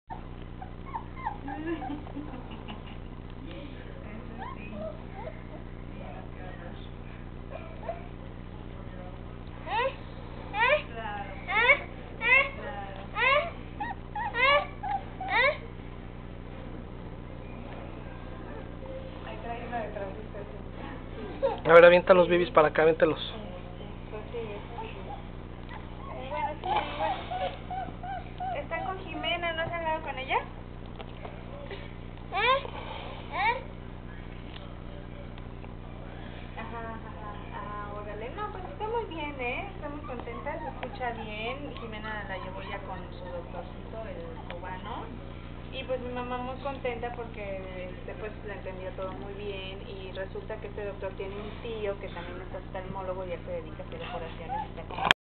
10 basset hound puppies and my daughter
4 weeks old the puppies